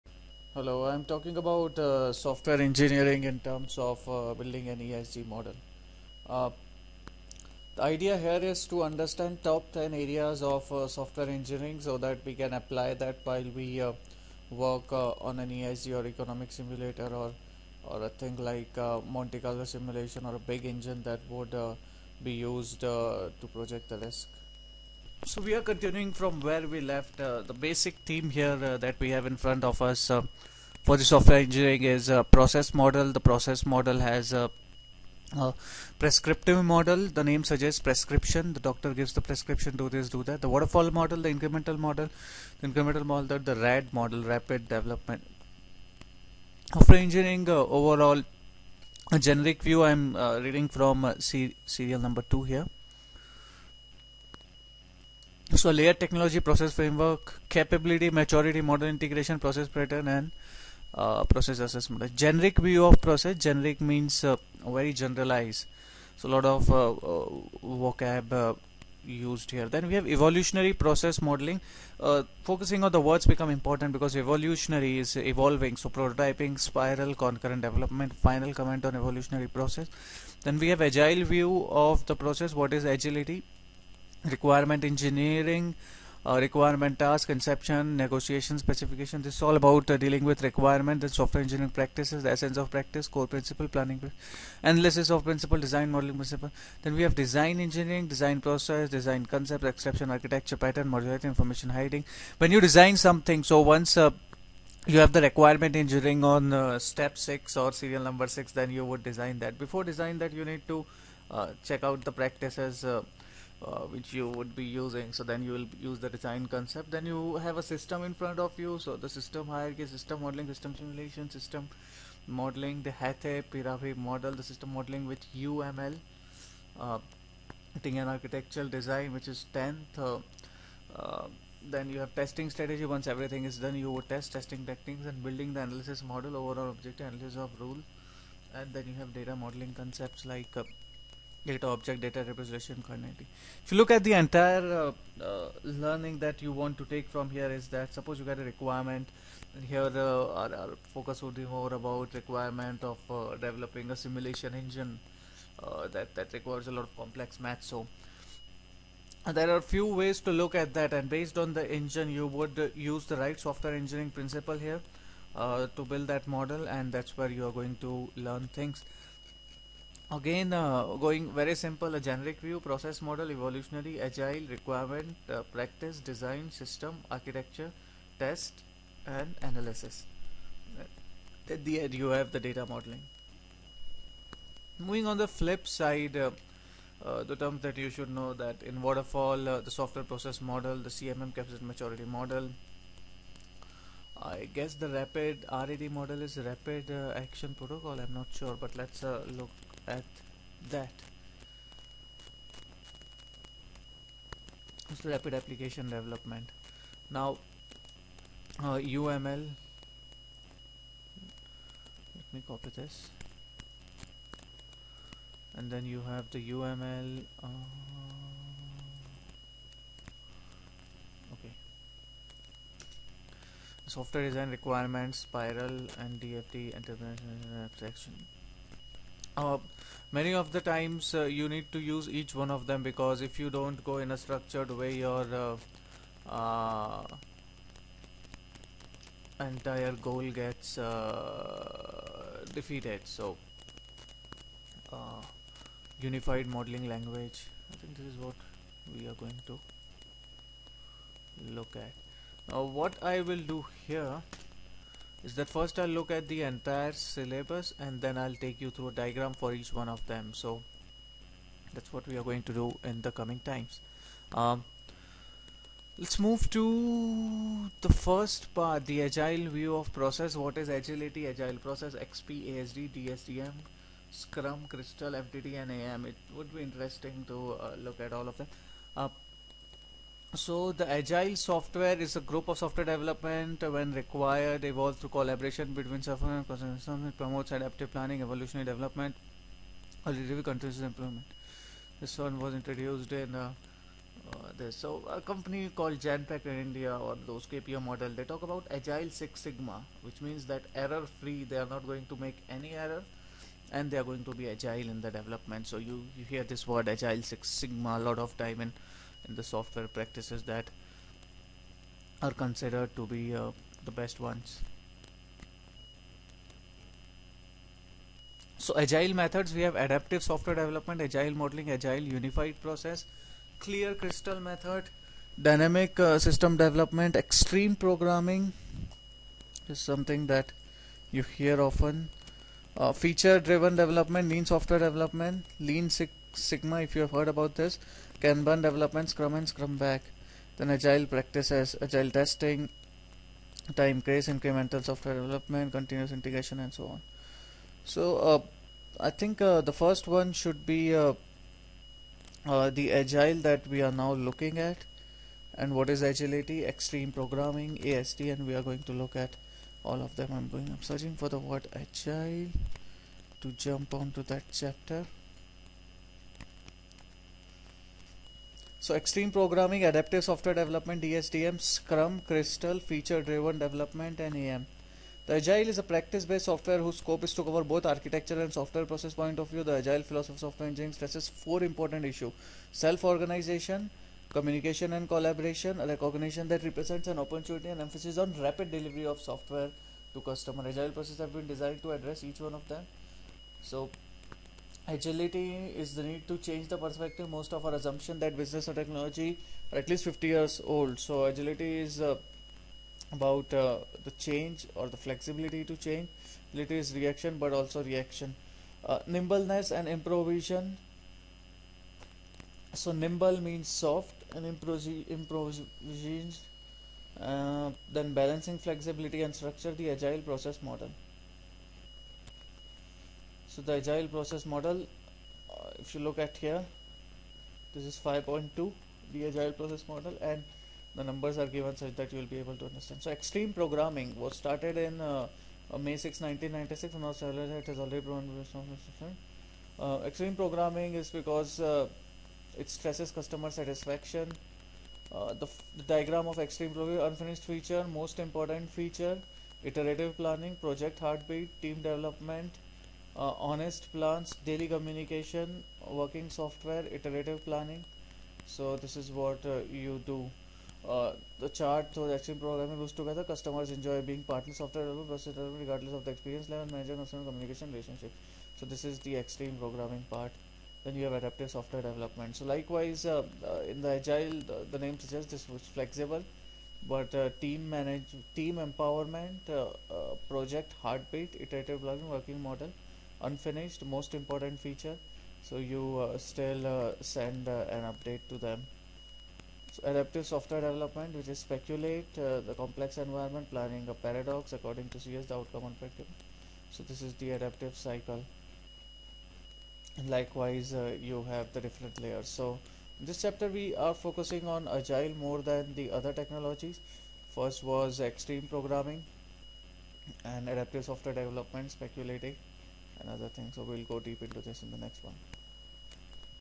Software Engineering - Introduction to syllabus
The course syllabus of software engineering has been heavily debated and still is Mathematics for Computing.